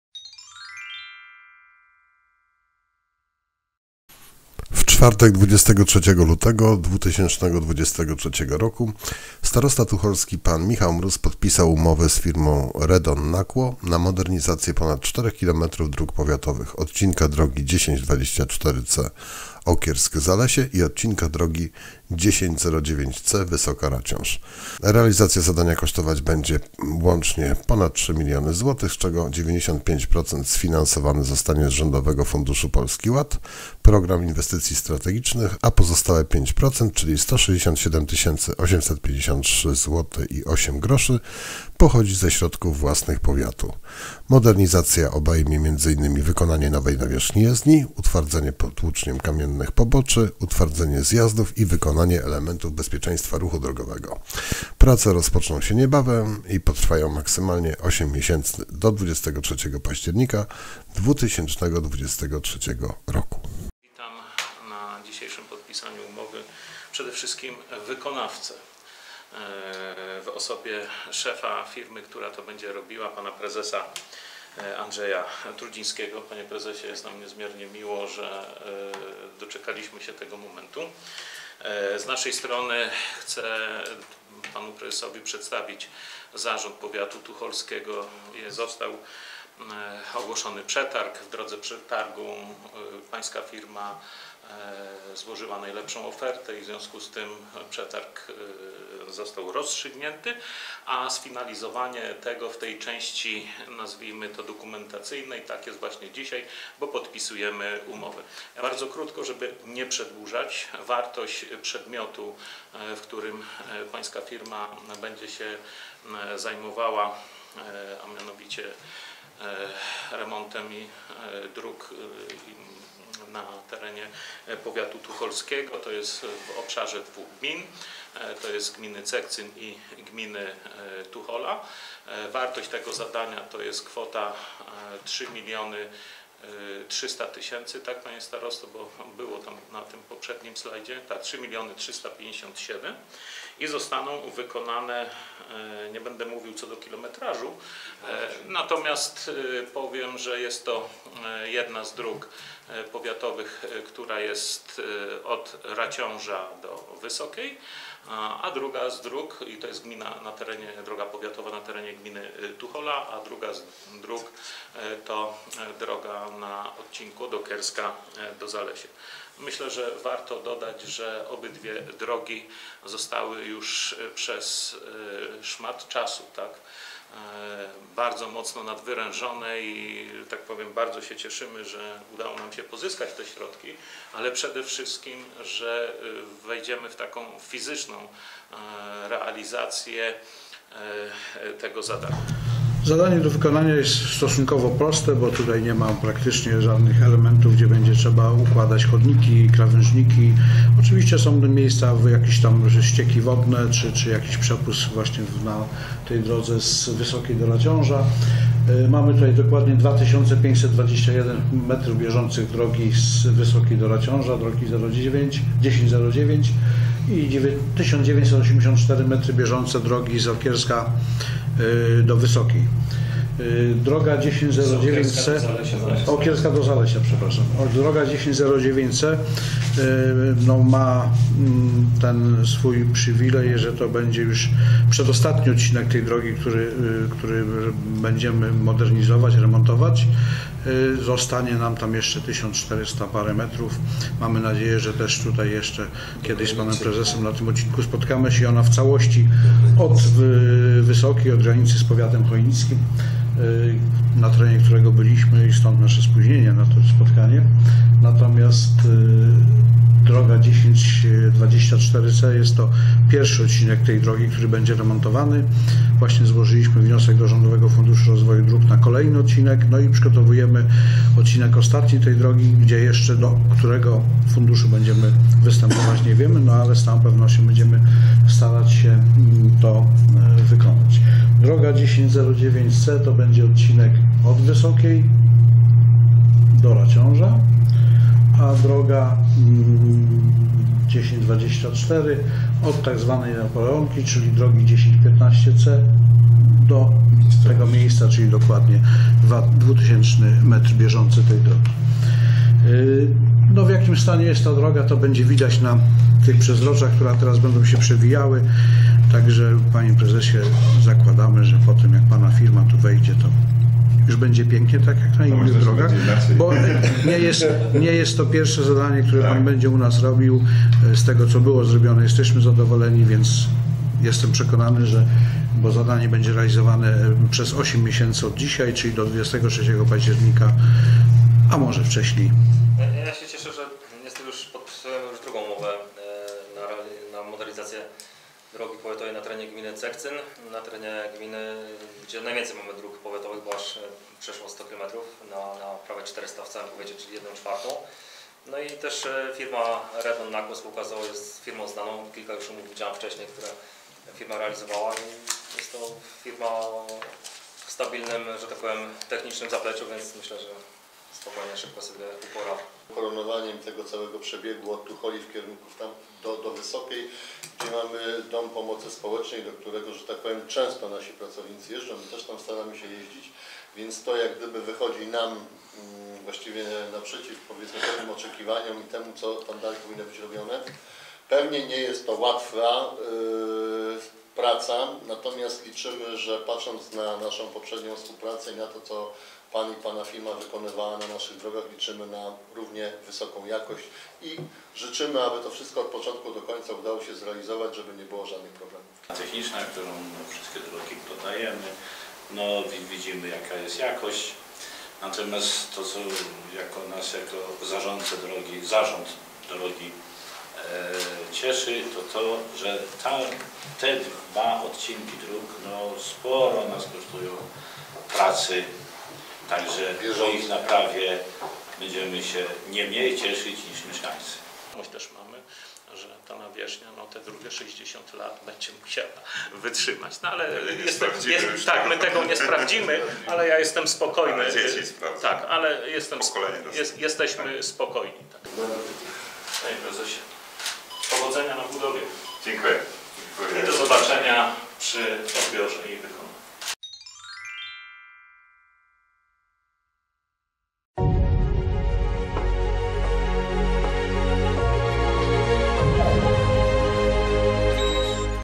Modernizacja dróg powiatowych w Gminie Cekcyn i Tuchola
W czwartek 23 lutego 2023 roku Starosta Tucholski Michał Mróz podpisał umowę z firmą REDON NAKŁO na modernizację ponad 4 km dróg powiatowych: odcinka drogi 1024C Okiersk-Zalesie i odcinka drogi 1009C Wysoka - Raciąż.